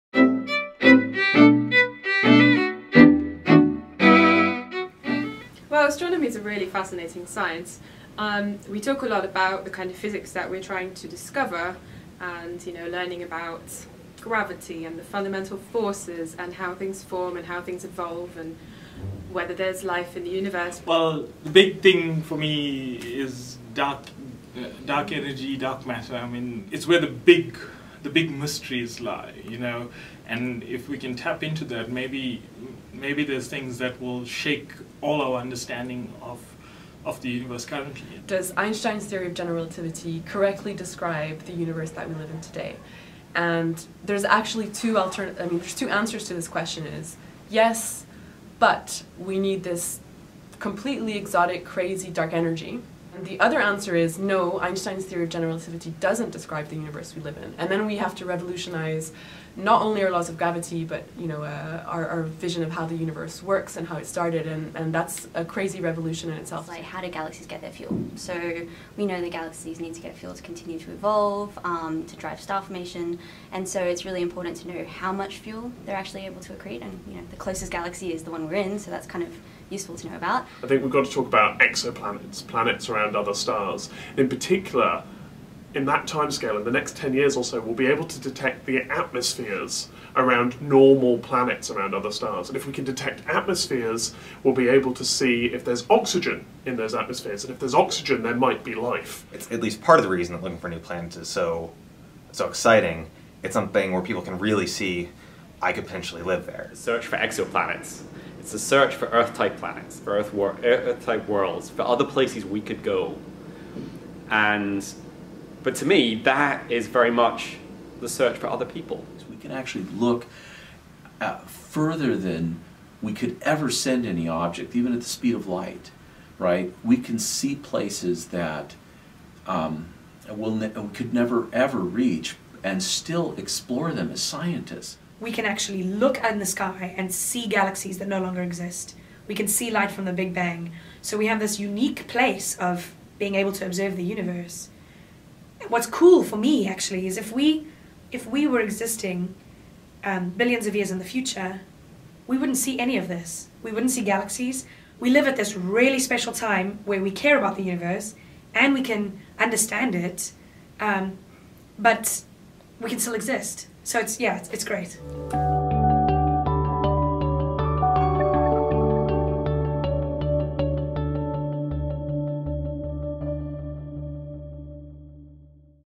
.astro5 - What's the Next Big Thing in Astronomy?
A short video asking dotAstronomy5 participants, "What big question do you want to see answered by astronomy?"